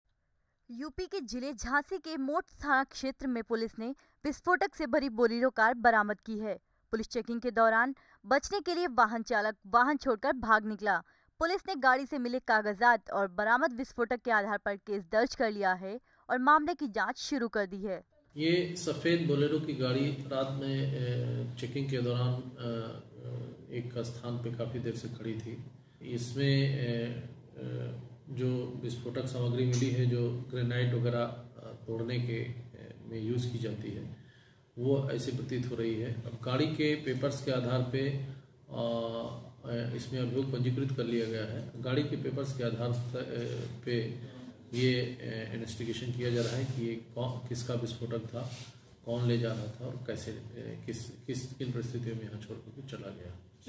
झांसी में विस्फोटक से भरी बोलेरो कार बरामद, तफ्तीश में जुटी पुलिस
झांसी : जनपद के मोठ थाना क्षेत्र में पुलिस ने विस्फोटक से भरा बोलेरो कार बरामद की है. भारी मात्रा में कार में भरा विस्फोटक कहां से कहां जा रहा था, यह अभी तक पता नहीं चल सका है. पुलिस चेकिंग के दौरान बचने के लिए वाहन चालक वाहन छोड़कर मौके से भाग निकला.